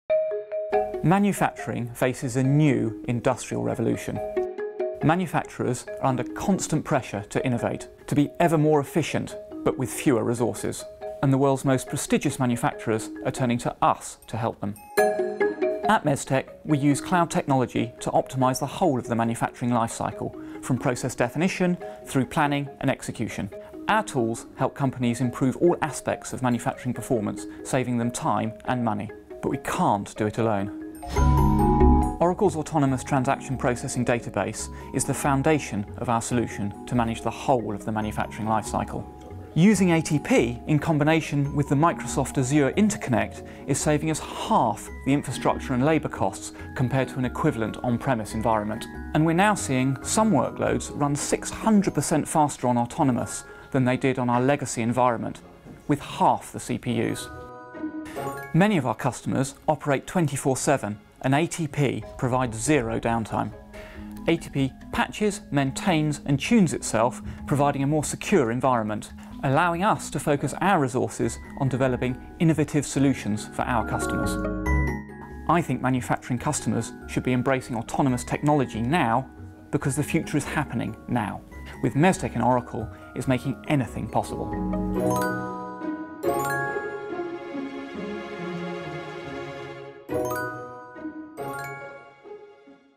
MESTEC Innovates Manufacturing with Autonomous Database
Using Oracle Autonomous Transaction Processing in combination with Microsoft Azure Interconnect has helped MESTEC cut its labor and infrastructure costs in half, and it is seeing workloads run up to 600% faster with half as many CPUs.